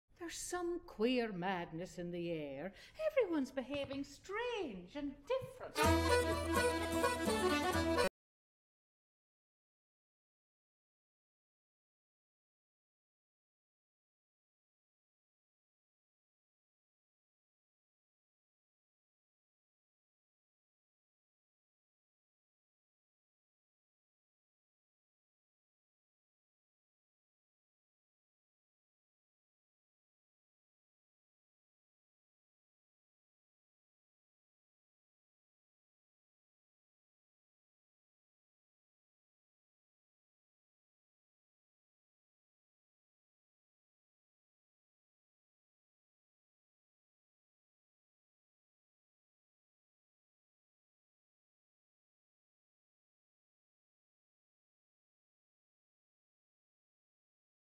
Drama at Inish | Show Trailer
Lennox Robinson's Drama at Inish

Lennox Robinson's much-loved farce from the Irish canon is directed by Cal McCrystal in a laugh-out-loud new production on the Abbey Stage this Winter.